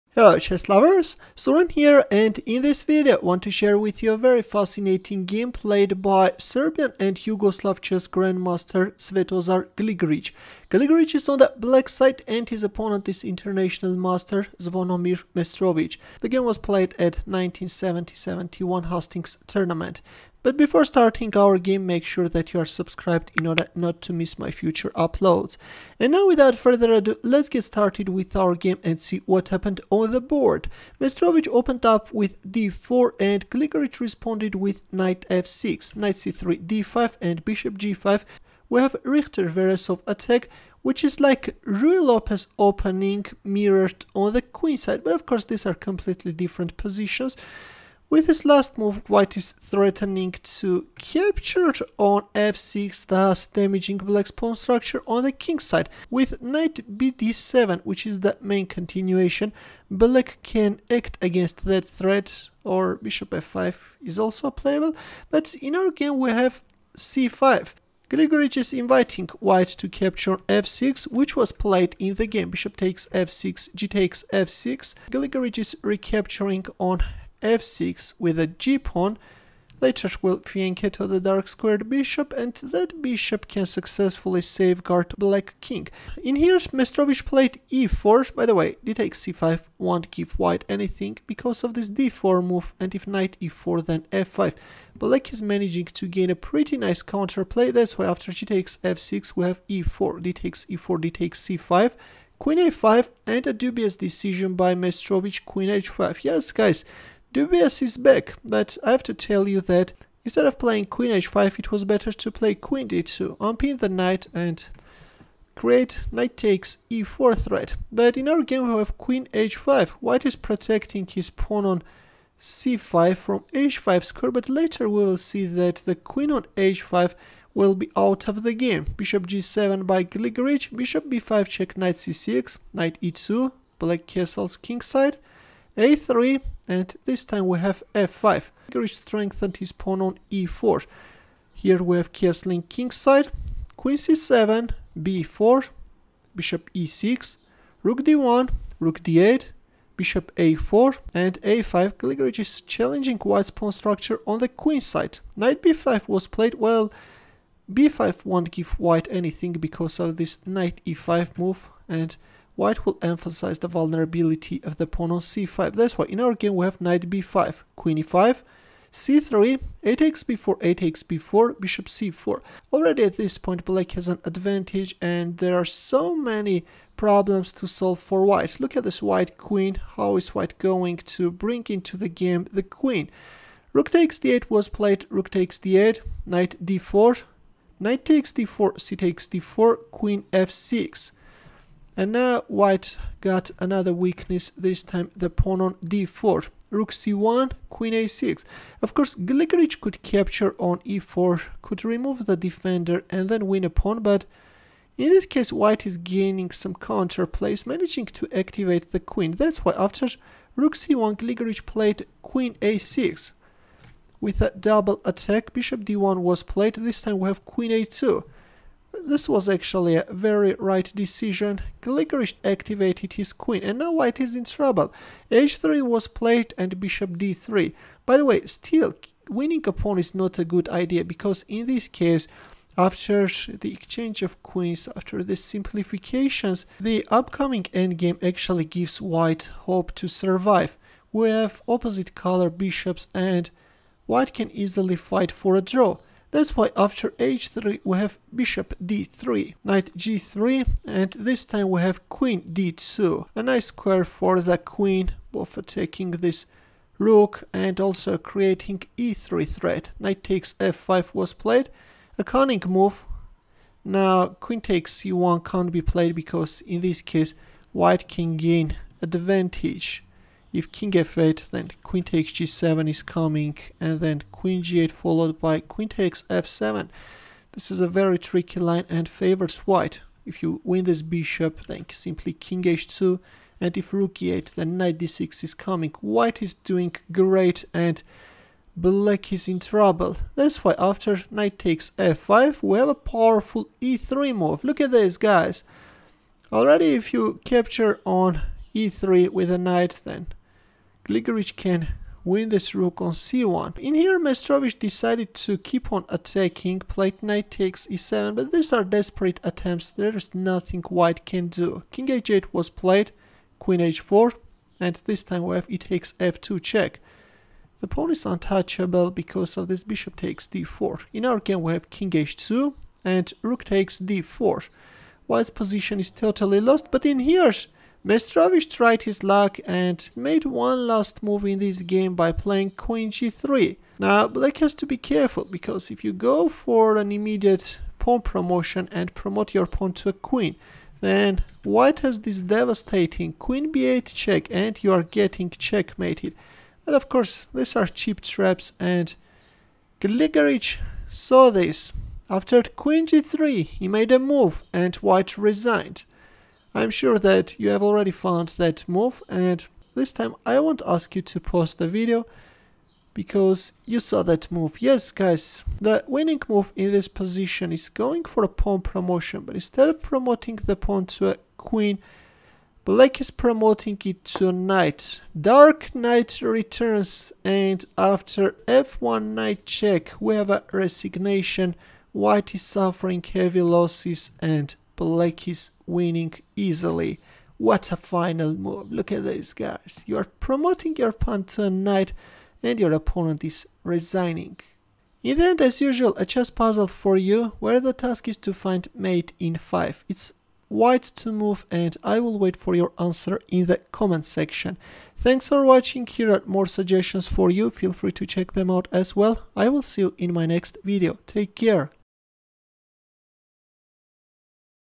The Dark Knight Returns
In this video I am analyzing a famous game played by Serbian and Yugoslav chess Grandmaster Svetozar Gligoric vs IM Zvonimir Mestrovic! In the opening White places his queen on an awkward h5-square and faces serious problems! While White queen was out of the game Gligoric activates his queen and gets a winning position! In the end Gligoric promotes his pawn to a knight, forks the royal family and forces his opponent to resign! In the end of the video as usual you can solve the daily puzzle!
Zvonimir Mestrovic vs Svetozar Gligoric
Hastings (1970/71), Hastings ENG, rd 9, Jan-07
Indian Game: General (A45)
1.d4 Nf6 2.Nc3 d5 3.Bg5 c5 4.Bxf6 gxf6 5.e4 dxe4 6.dxc5 Qa5
7.Qh5 Bg7 8.Bb5+ Nc6 9.Ne2 O-O 10.a3 f5 11.O-O Qc7 12.b4 Be6
13.Rad1 Rad8 14.Ba4 a5 15.Nb5 Qe5 16.c3 axb4 17.axb4 Bc4
18.Rxd8 Rxd8 19.Nbd4 Nxd4 20.cxd4 Qf6 21.Rc1 Qa6 22.Bd1 Qa2
23.h3 Bd3 24.Ng3 Qd2 25.Nxf5 e3 26.Nxe7+ Kh8 27.Qh4 exf2
28.Kh2 Rxd4 29.Qg3 f1=N+
0-1